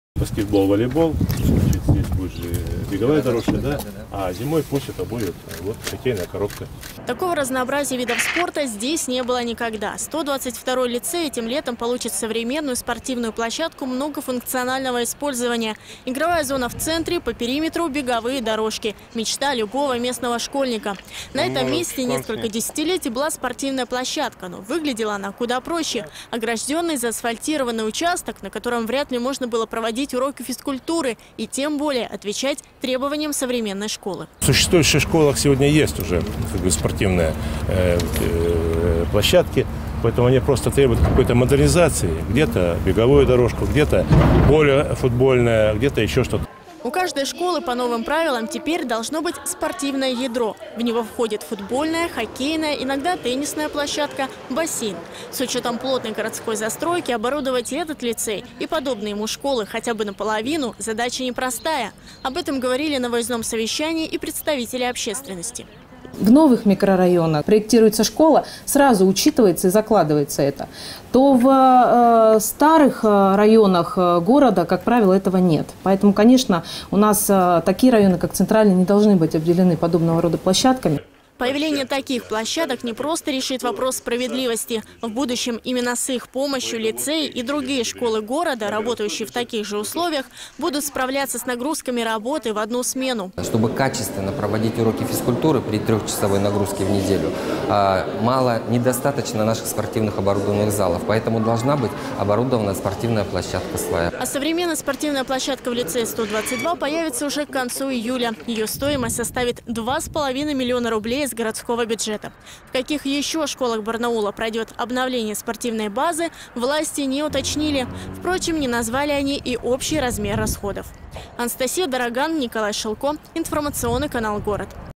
Сергей Дугин: Спортивные площадки барнаульских школ ждет обновление
Выездное совещание прошло в одном из инновационных лицеев и было посвящено реализации городского проекта «Современная школа», который выдвигает очень жесткие требования к общеобразовательным учреждений.